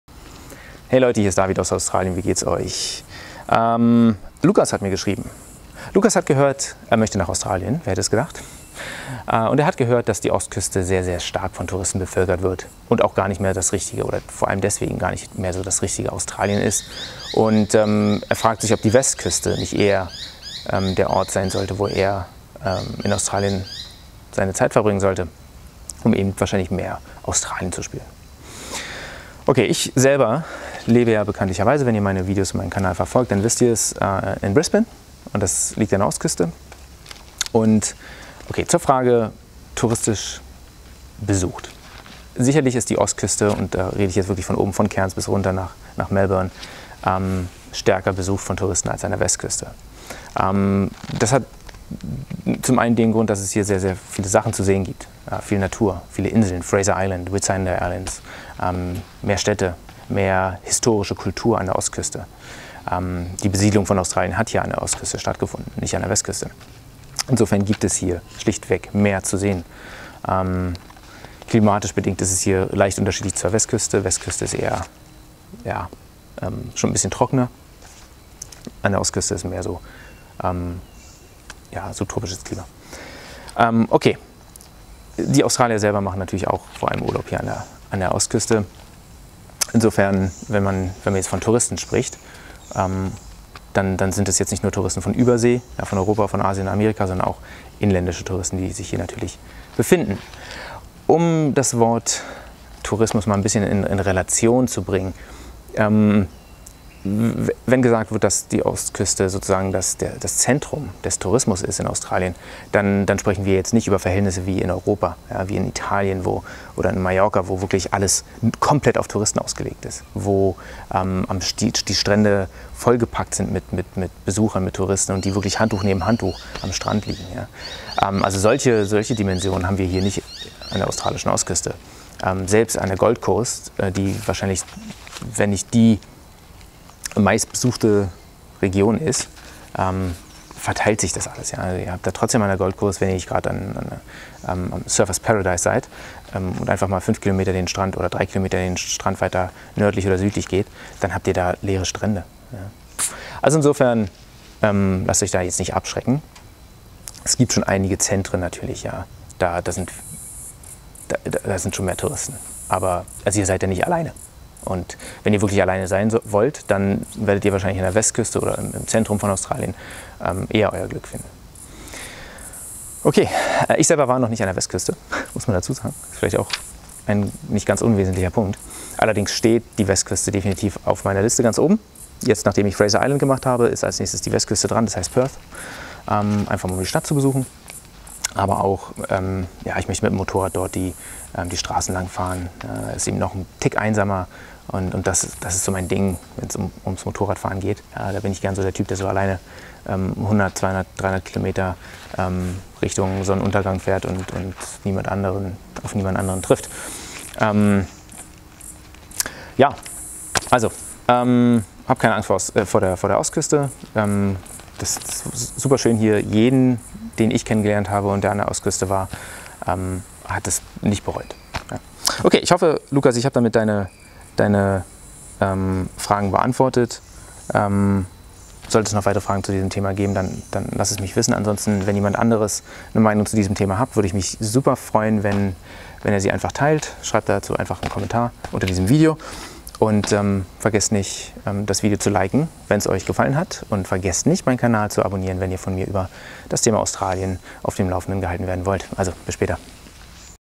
Australien - Westküste besser als Ostküste?
Sollte man eher zur Ostküste oder Westküste von Australien? Hier ein  paar Gedanken von mir.